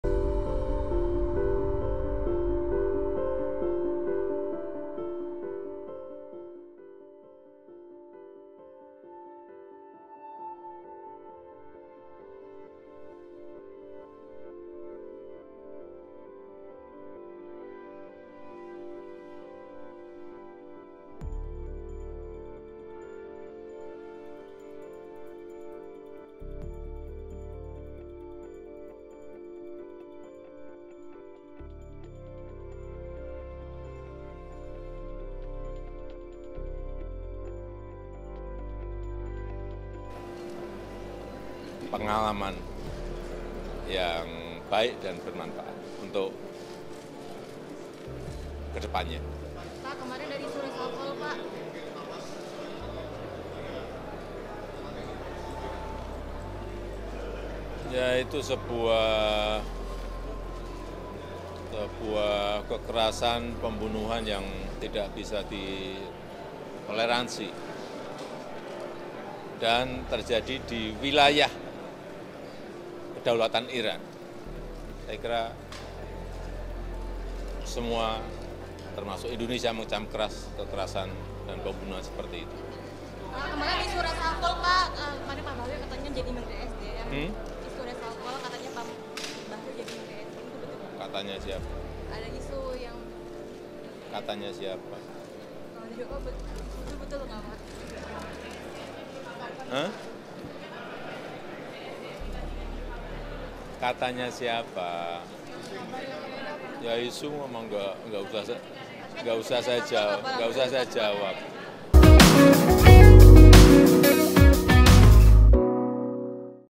Tanggapan Presiden Jokowi Soal Tewasnya Pemimpin Hamas
Presiden Joko Widodo menanggapi terkait pembunuhan pemimpin Hamas, Ismail Haniyeh.

Jokowi menyebut pembunuhan tersebut adalah kekerasan yang tidak dapat ditoleransi.

Hal itu diungkapkan Presiden Jokowi di Jakarta Convention Center, Jakarta, Kamis (1/8/2024).

Diberitakan sebelumnya, insiden tragis itu terjadi di Ibu Kota Iran, Rabu (31/7/2024).
Video terkini dan terbaru..